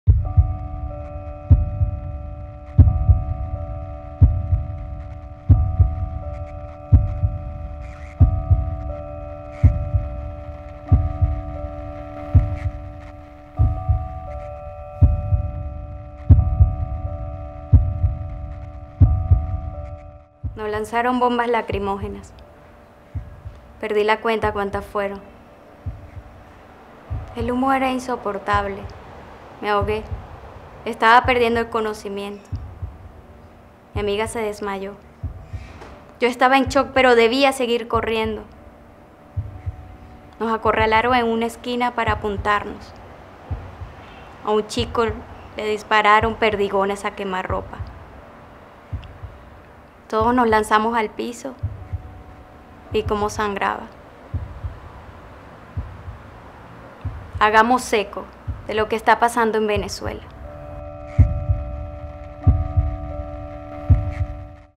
Greysi Mena. Eco a una manifestante testigo. 22-02-2014. Venezuela.
La actriz hace eco desde Venezuela de lo que vio una manifestante y narró desde su twitter @Mcbzovko el pasado 22 de febrero. Hagamos ECO de lo que está pasando en Venezuela.